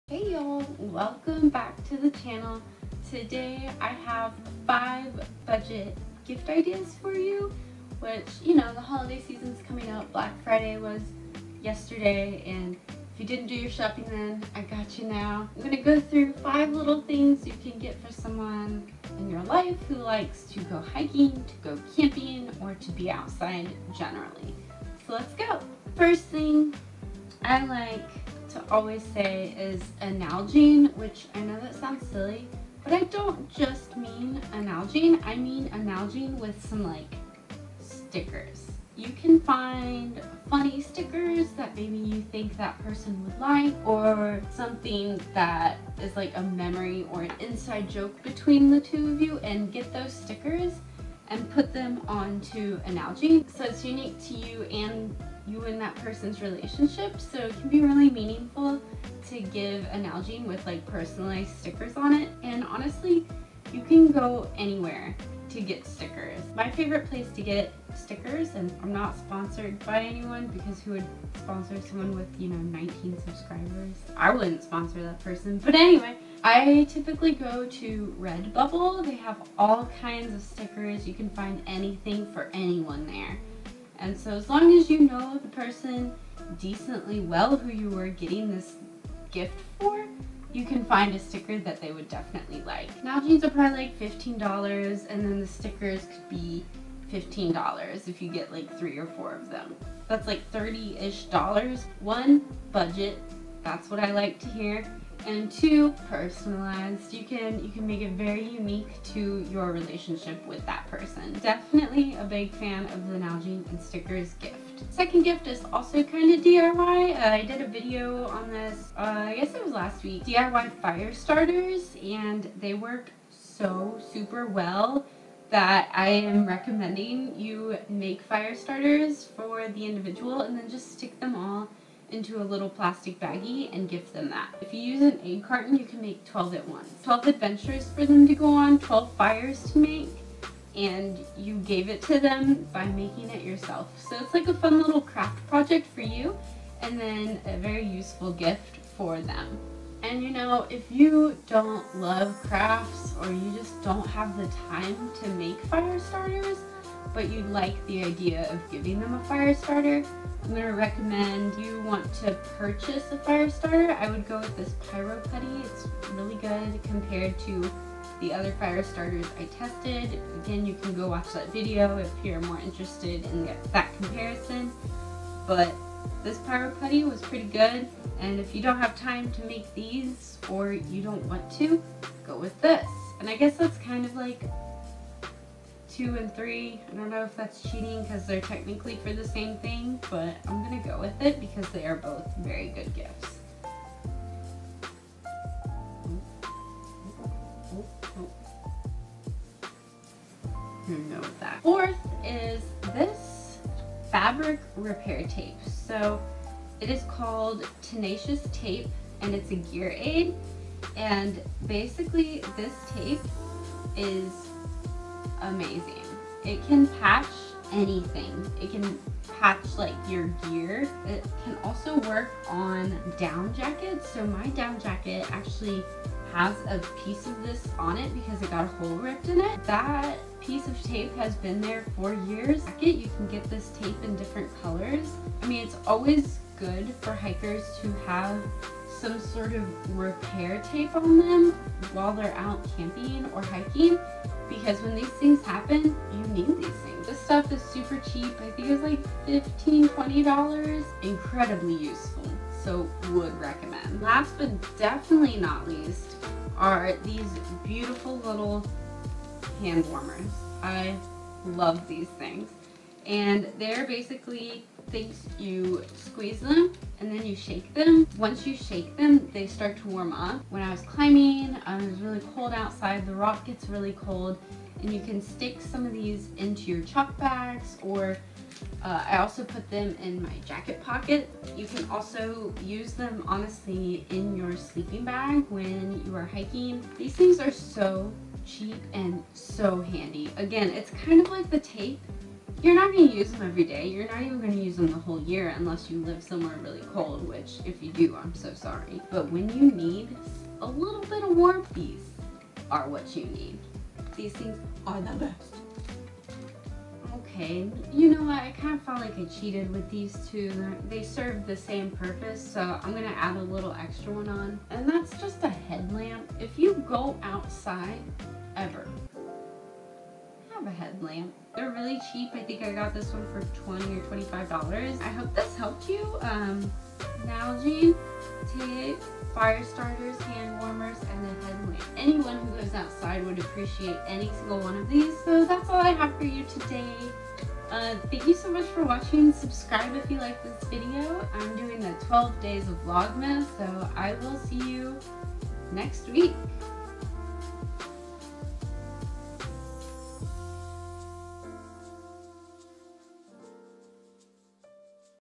BUDGET GIFT IDEAS FOR OUTDOORSY FOLK // Best gifts under $30 for hikers, campers, and backpackers
Looking for some unique gift ideas for hikers, campers, or backpackers in your life? Well, here are my top five (plus a little bonus gift) gifts under $30 that you can get for anyone who likes the outdoors! These gifts are simple but are so important to anyone going outdoors and can be appreciated by anybody who hikes, camps, or goes backpacking.